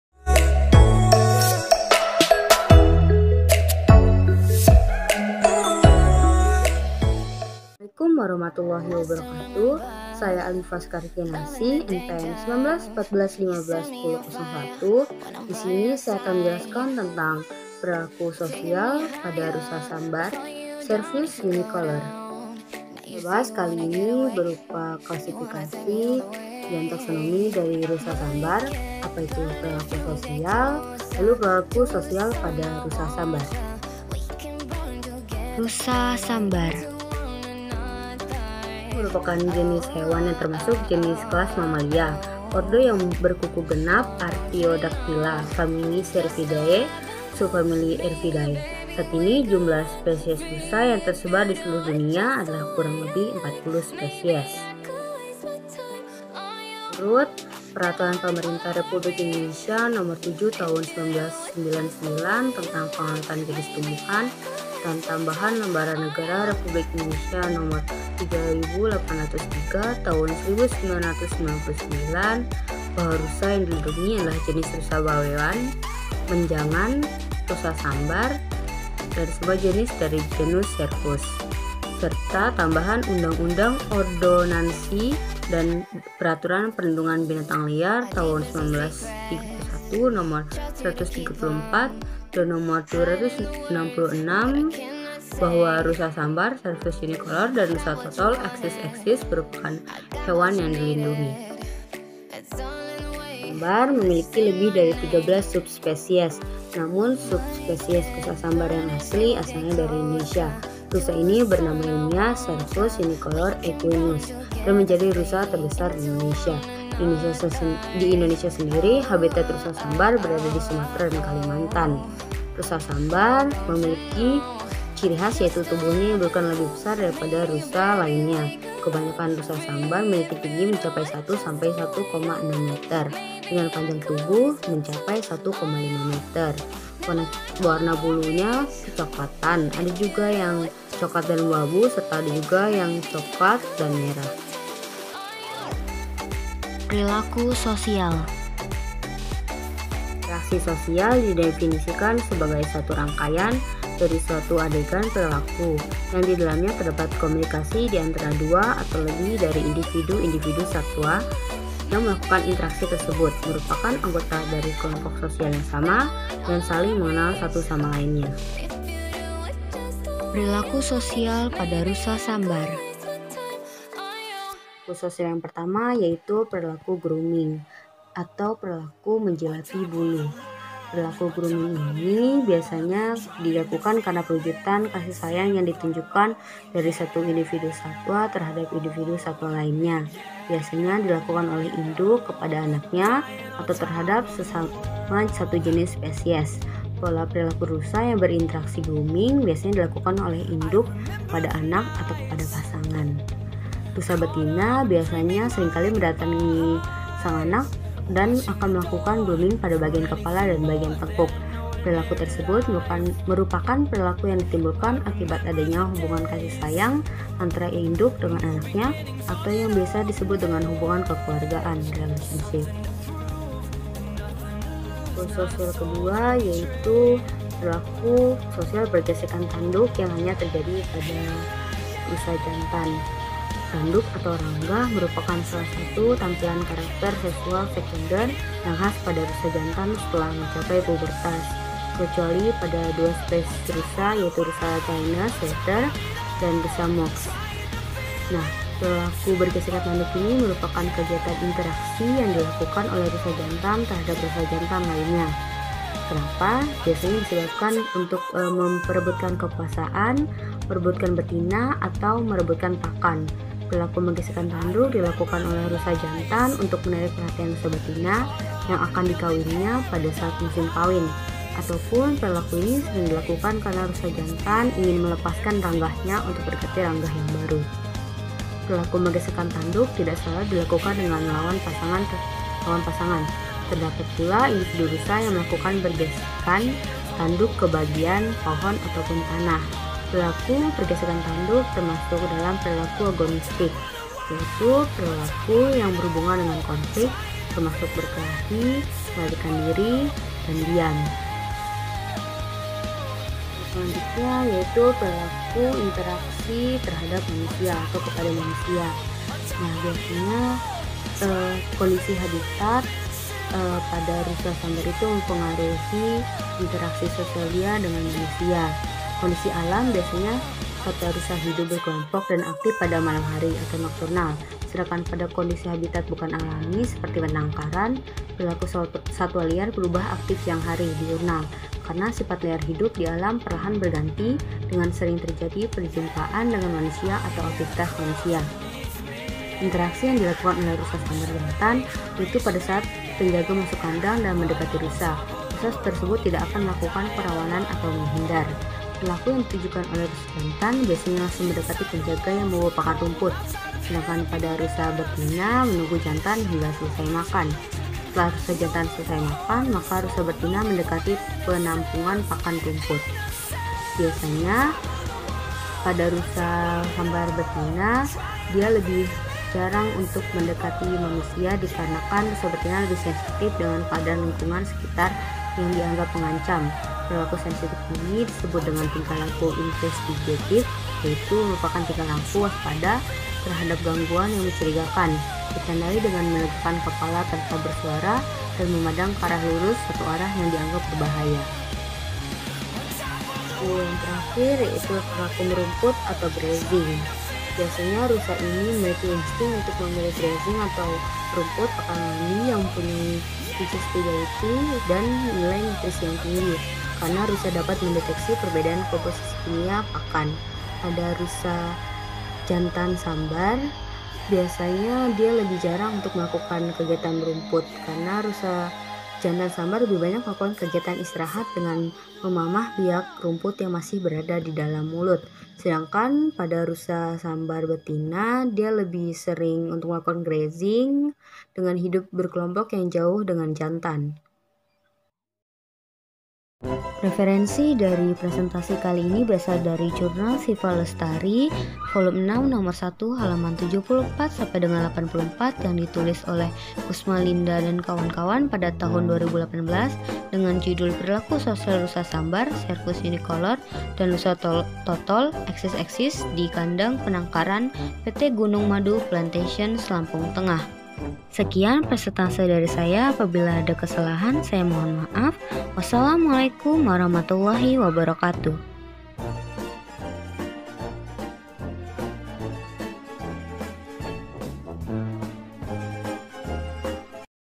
Perilaku Sosial pada Rusa Sambar (Cervus unicolor)
Tugas Mata Kuliah Perilaku Satwa Liar
Nama: Aliffa Sekar Kinasih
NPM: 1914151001
Jurusan: Kehutanan
Fakultas: Pertanian
Universitas Lampung.